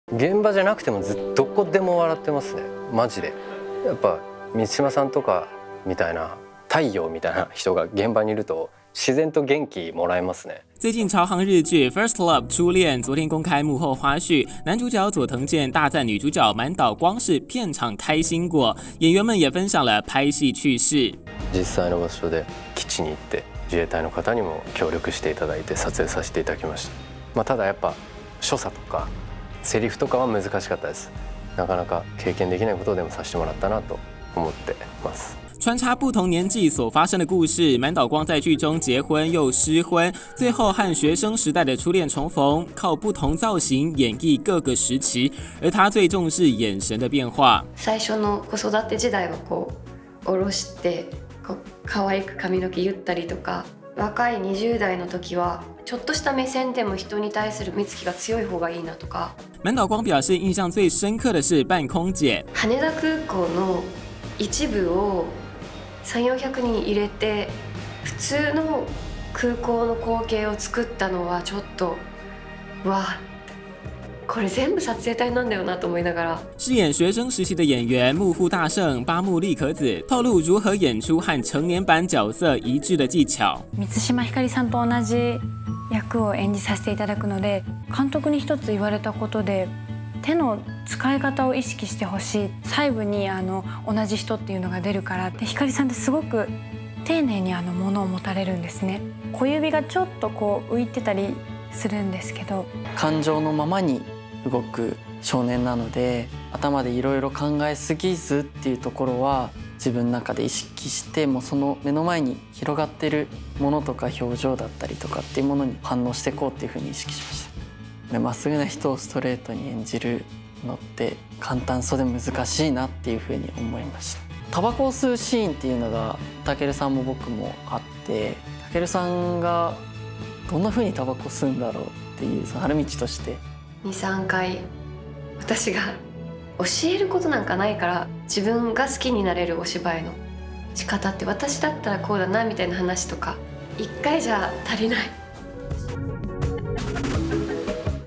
《First Love 初戀》滿島光「扮空姐」印象最深刻 佐藤健讚她愛笑「是大家的太陽」｜鏡速報
最近超夯日劇《First Love 初戀》昨天 （19日）公開幕後花絮，男主角佐藤健大讚女主角滿島光是片場開心果。佐藤健分享最難拍的戲是「開戰鬥機」，他透露「開飛機的手勢還有口令是最困難的」。女主角滿島光表示印象最深刻的是「扮空姐」，她在劇中結婚又失婚，最後和學生時代的初戀重逢，靠不同造型演繹各個時期，最重視「眼神」的變化。